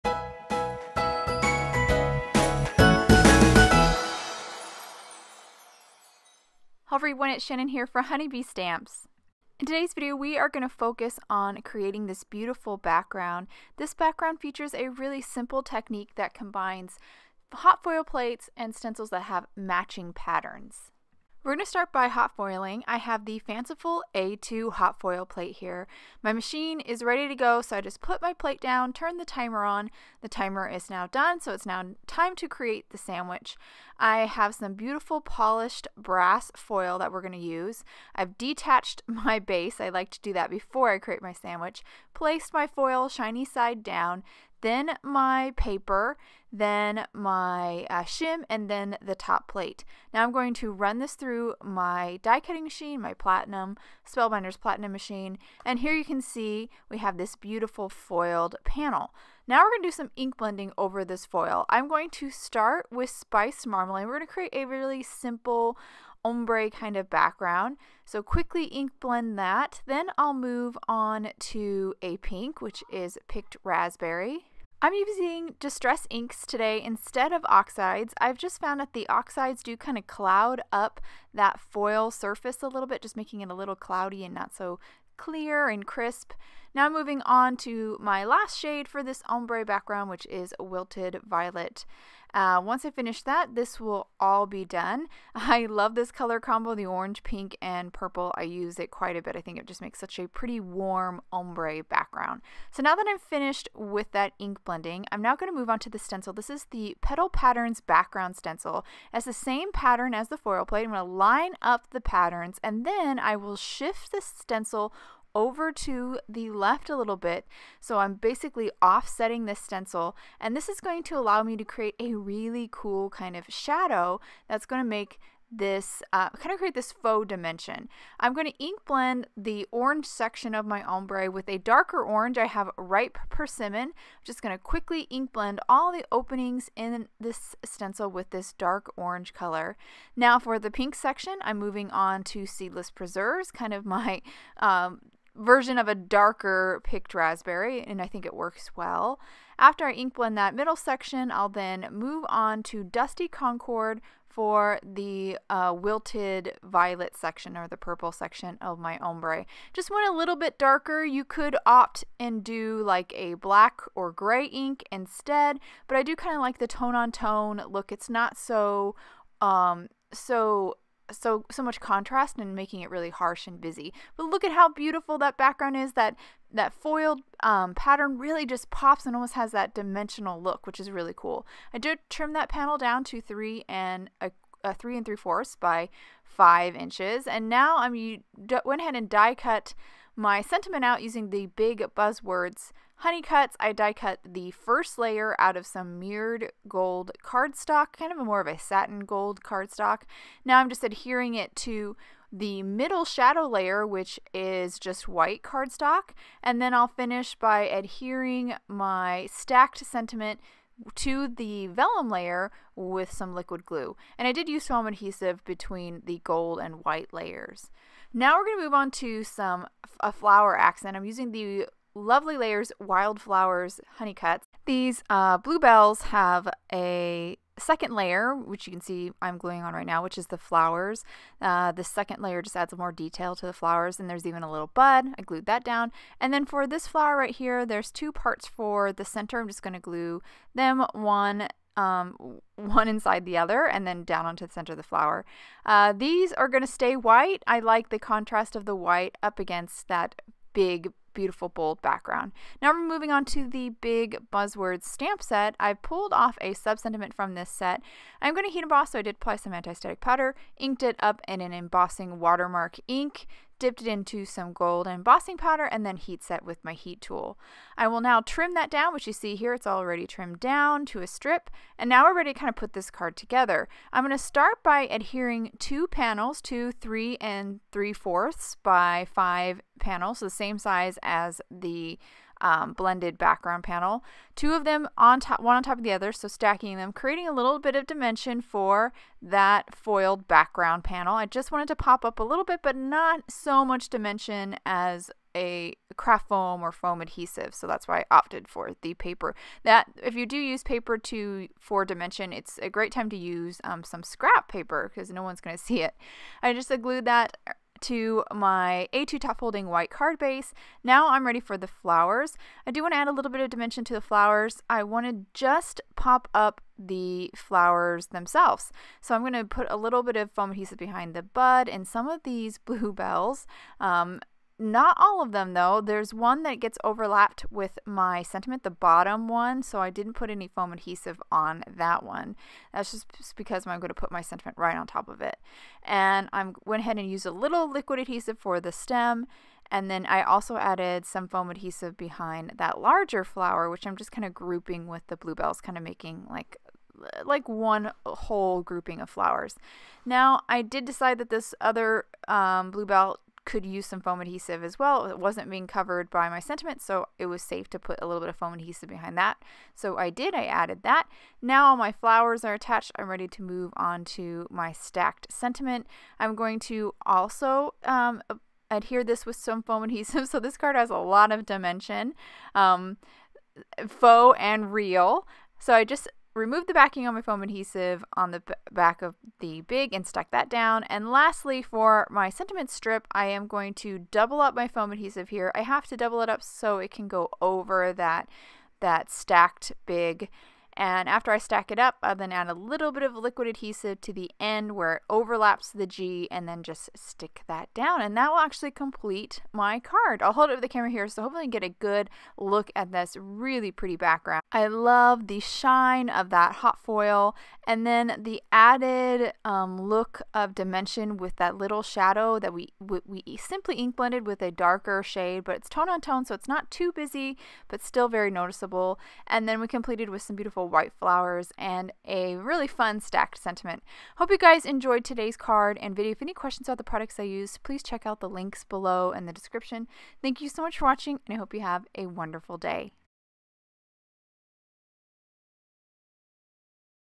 Foil Plates with Matching Stencils
In this video Channin creates a beautiful birthday card featuring the Fanciful A2 Foil Plate and Petal Patterns Background Stencil. Watch to see how she adds faux dimension to the hot foiled panel with a simple technique.
HoneyBeeStamps.com

• Craft Perfect | Satin Mirror Card | 8.5x11 5/pkg | Gold Pearl

• My Sweet Petunia | NEW Original MISTI | Laser Etched | Stamping Tool

• WOW! Embossing Powder | Metallic Gold Rich